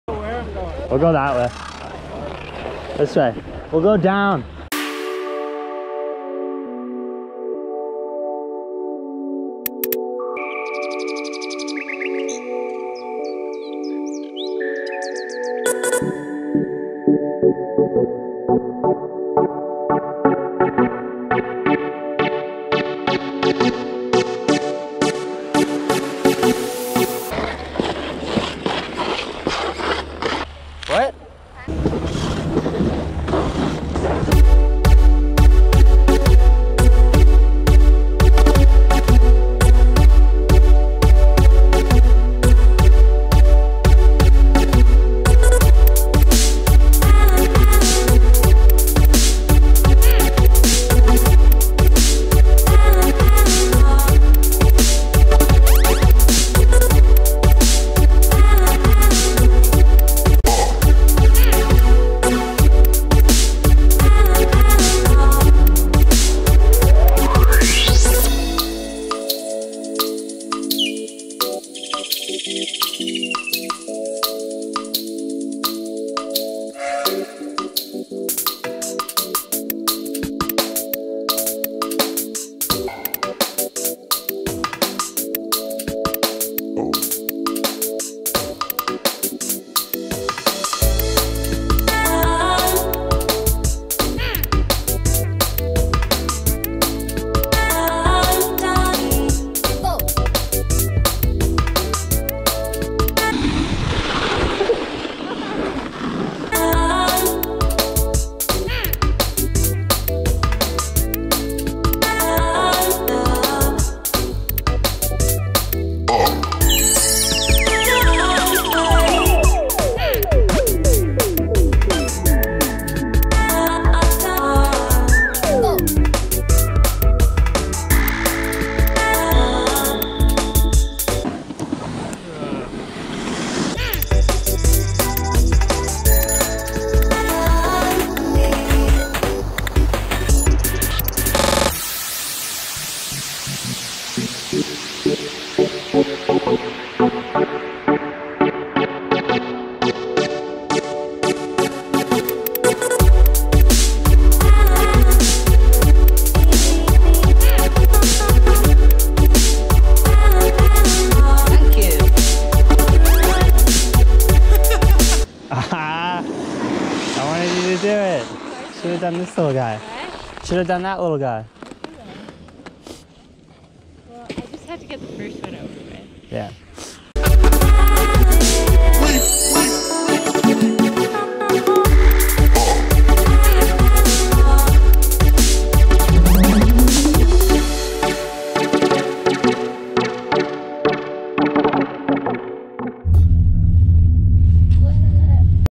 Ski Lake Louise Episode 1
Skiing at the world famous Lake Louise Ski Resort in Banff National Park, Alberta Canada.

Videos Recorded on Go Pro Hero 7 Black

Like the Music check The artist out:

Soundcloud.com/jamesreesor

Soundcloud.com/danger_dave